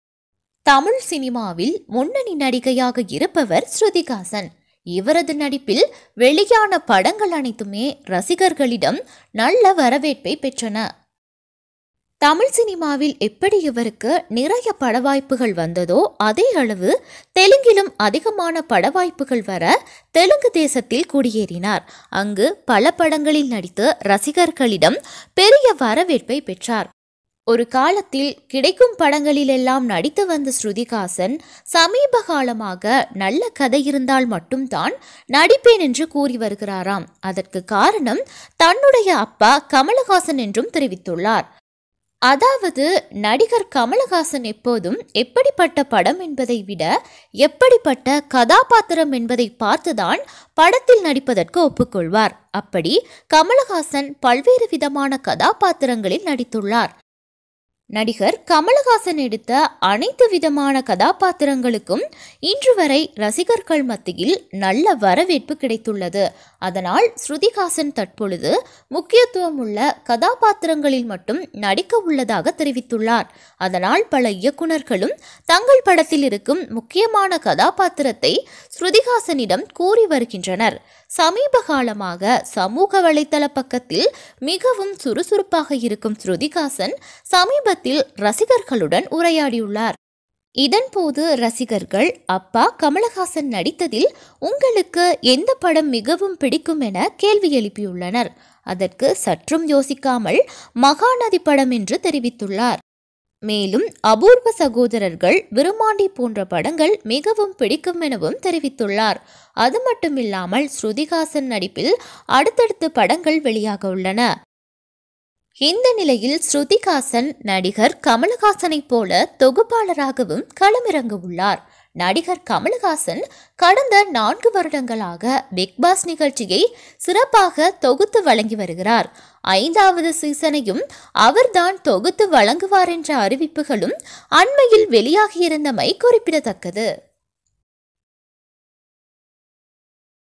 அப்பா இப்படிதான்.... ஆண்டவரின் மகள் ஸ்ருதிஹாசன் எடுத்த அதிரடி முடிவு
தமிழ் சினிமாவில் முன்னணி நடிகையாக இருப்பவர் ஸ்ருதிஹாசன். இவரது நடிப்பில் வெளியான படங்கள் அனைத்துமே ரசிகர்களிடம் நல்ல வரவேற்பைப் பெற்றன..

 எப்படி தமிழ் சினிமாவில் இவருக்கு நிறைய படவாய்ப்புகள் வந்ததோ அதே அளவு தெலுங்கிலும் அதிகமான பட வாய்ப்புகள் வர தெலுங்கு தேசத்திற்கு குடியேறினார்.

அங்கு பல படங்களில் நடித்து ரசிகர்களிடம் பெரிய வரவேற்பைப் பெற்றார். ஒரு காலத்தில் கிடைக்கும் படங்களில் எல்லாம் நடித்து வந்த ஸ்ருதிஹாசன் சமீபகாலமாக நல்ல கதை இருந்தால் மட்டும்தான் நடிப்பேன் என கூறி வருகிறாராம் அதற்கு காரணம் தன்னுடைய அப்பா எனவும் தெரிவித்துள்ளார்.

அதாவது கமல்ஹாசன் எப்போதுமே எப்படிப்பட்ட படம் என்பதை விட எப்படிப்பட்ட கதாபாத்திரம் என்பதை பார்த்து தான் படத்தில் நடிப்பதற்கு ஒப்புக்கொள்வார்.

அப்படி கமல்ஹாசன் பல்வேறு விதமான கதாபாத்திரங்களில் நடித்துள்ளார். அதில் அனைத்து கதாபாத்திரங்களும் ரசிகர்கள் கொண்டாடினர்.

அதனால் ஸ்ருதிஹாசன் தற்போது முக்கியத்துவம் உள்ள கதாபாத்திரத்தில் மட்டும் நடிக்க உள்ளதாக தெரிவித்துள்ளார்.

அதனால் பல இயக்குனர்களும் தங்கள் படத்தில் இருக்கும் முக்கியமான கதாபாத்திரத்தை ஸ்ருதி ஹாசனிடம் கூறி வருகின்றனர். சமீபகாலமாக சமூக வலைதளப் பக்கத்தில் ஆக்டிவாக இருக்கும் சுருதிஹாசன் சமீபத்தில் ரசிகர்களுடன் உரையாடியுள்ளார்.

அப்போது ரசிகர்கள் பல்வேறு விதமான கேள்விகள் எழுப்ப ஒரு ரசிகர் உங்கள் அப்பா கமல்ஹாசன் நடித்ததில் உங்களுக்கு எந்த படம் ரொம்ப பிடிக்கும் என கேள்வி கேட்டுள்ளார். அதற்கு சற்றும் யோசிக்காமல் மகாநதி படம் என தெரிவித்துள்ளார். மேலும் அபூர்வ சகோதரர்கள் மற்றும் விருமாண்டி போன்ற படங்கள் ரொம்ப பிடிக்கும் எனவும் தெரிவித்துள்ளார். அதுமட்டுமில்லாமல் ஸ்ருதிஹாசன் நடிப்பில் அடுத்தடுத்து படங்கள் வெளியாக உள்ளன.

,இந்த நிலையில் நடிகர் கமலை போல தொகுப்பாளராக வும் களமிறங்க உள்ளார். நடிகர் கமல் கடந்த 4 வருடங்களாக பிக் பாஸ் நிகழ்ச்சியை சிறப்பாக தொகுத்து வழங்கி வருகின்றார்.5 வது சீசனையும் அவர் தான் தொகுத்து வழங்குவார் என்ற அறிவிப்புகளும் வெளியாகியிருந்தமை குறிப்பிடத்தக்கது.